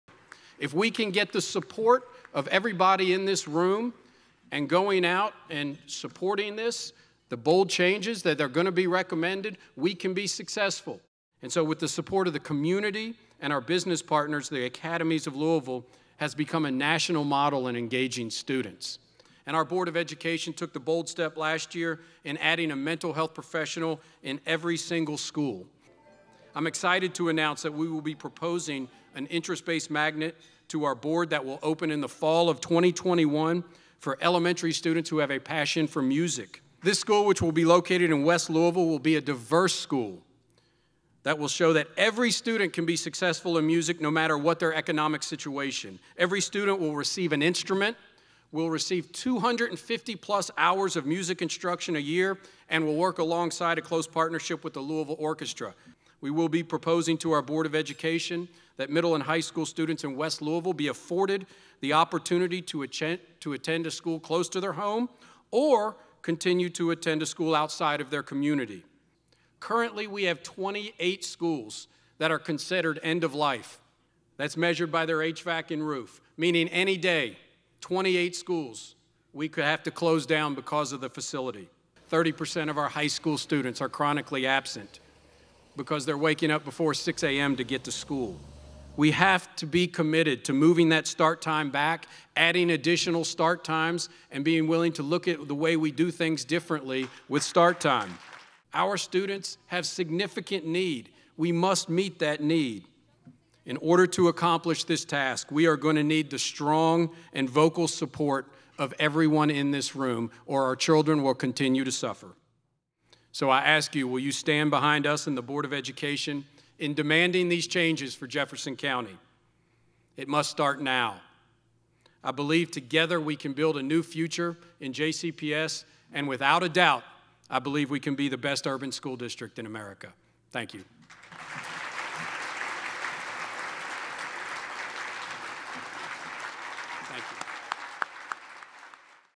JCPS State of District 2020 -- Dr. Pollio
Jefferson County Public Schools (JCPS) must build on its momentum of the past two years and implement bold changes to ensure the district continues to move forward and engage students, according to JCPS Superintendent Dr. Marty Pollio.

“Our district is improving and growing like never before,” Dr. Pollio told the more than 300 attendees during his second State of the District Address.  “We are implementing new projects and launching bold initiatives while addressing more than 270 areas identified in our Corrective Action Plan. But if we want to continue to be a leader in urban education and see groundbreaking changes in student engagement, we have to fundamentally change the way we do business to serve generations to come.”

Dr. Pollio said that means ensuring all students have access to modern, engaging learning environments; providing more resources for the district’s most vulnerable students; hiring teachers and administrators who more accurately reflect the student population; and fostering community involvement.

Noting that schools are some of the most visible symbols of what’s valued by a society, Dr. Pollio called for more community investment in JCPS, adding that Anchorage, Fayette, Oldham, Nelson and Henry school districts all have higher property tax rates, while serving a fraction of the students that JCPS serves.  “What is the difference between our county and others?” he asked. “Investment.”

In addition, Dr. Pollio said a thriving community must be diverse, but there must also be choice.

“We are proposing to our board of education that middle and high school students in West Louisville be afforded the opportunity to choose to attend school in their community, or continue to attend school outside of their neighborhood,” he said. “We want to give families the power to make the decision about what’s best for their child.”

Dr. Pollio also stressed the importance of expanding magnet options for students. Elementary students interested in music could be attending a new magnet program in West Louisville in 2021. Each student would be given an instrument, receive 250 hours of music instruction and have the opportunity to play alongside the Louisville Orchestra and conductor Teddy Abrams under Dr. Pollio’s proposal.

In addition, Dr. Pollio said JCPS is exploring ways to modernize instructional time for students. “Research on this is clear: there is no benefit to students for starting school before 8 a.m.,” he said.  “Moving back the start time of school could save money, increase the likelihood that students will attend, and ensure they’re ready to engage when they arrive.”

JCPS is also utilizing other innovative strategies to cultivate instructional time beyond the traditional school year, he said, such as the expansion of the Backpack League, the ambitious summer learning program launched last year.

Dr. Pollio said that as JCPS reaches more students with added instructional time, the district must enhance support for its most vulnerable students.  With nearly 6,000 students who are homeless and more than 60,000 who on free and reduced lunch, “students are coming to us with more needs than ever before and we have to make sure the leaders in our highest-need schools are supported,” he said, including stipends for teachers in these schools and technology enhancements.

Pollio also said that the development of a strong workforce and leadership team is essential to seeing success in JCPS.  He said the Louisville Teacher Residency Program, a partnership with the University of Louisville, “will be a game changer.”

“We believe this could be an opportunity to attract more minorities to the profession,” he said, adding that 37 percent of JCPS students are African American while only 12.6 percent of teachers are black.  “Setting the foundation to build a strong workforce is key.  We want to reach people at every level if there is interest in getting into education.”

Finally, he said JCPS is creating opportunities for the community to engage in schools, including expanding opportunities to mentor students who are preparing to present their Backpack defenses.

“It will take all of us investing in these things to make the impact we want to see and engage students in a new way,” he said. “This matters for every resident – in order to build a strong city it starts with public education.”
jefferson.kyschools.us